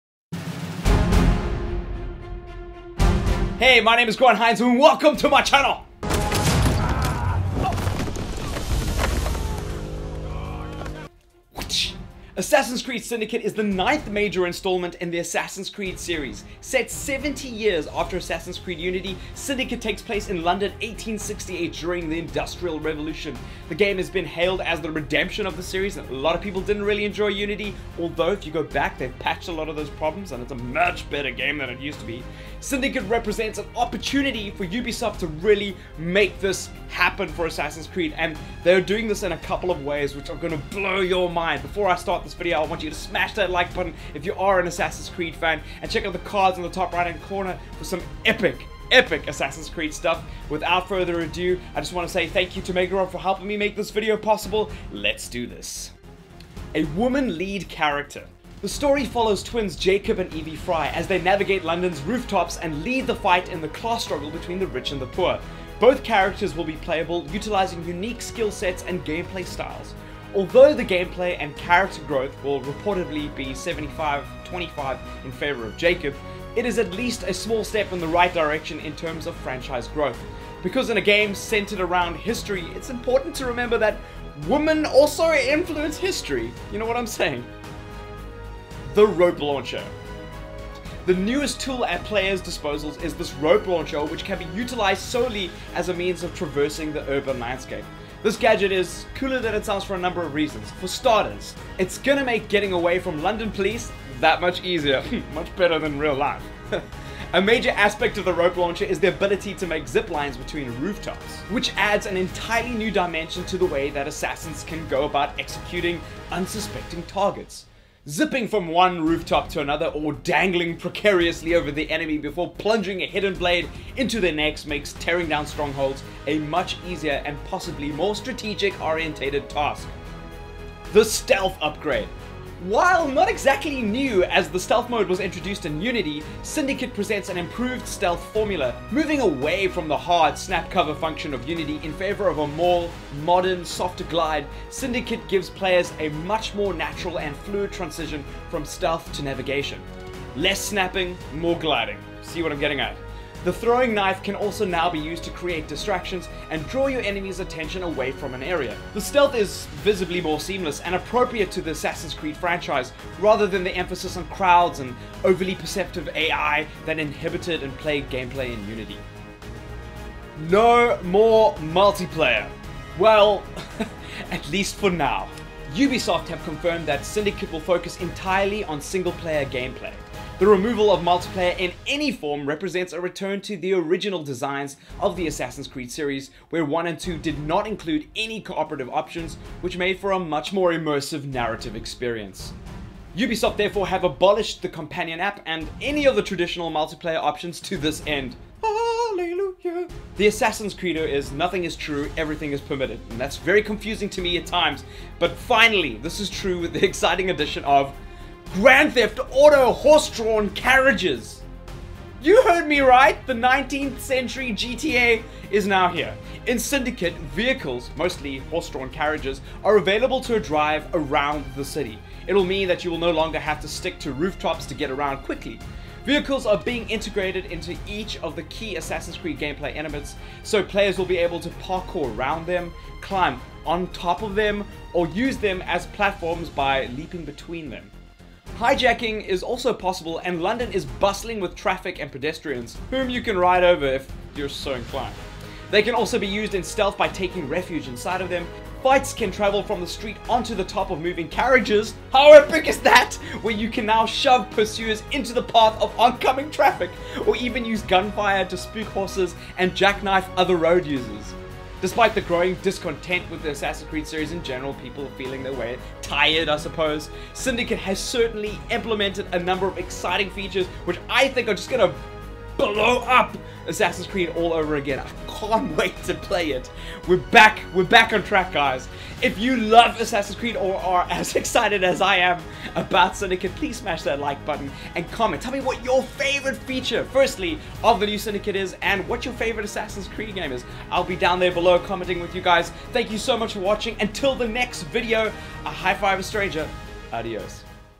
ASSASSIN'S CREED SYNDICATE: 5 Reasons To Get HYPED!
ASSASSIN'S CREED SYNDICATE! Let's get HYPED! Here are 5 reasons to get on that Hype Train! COMMENT WITH YOUR FAVOURITE THING ABOUT SYNDICATE!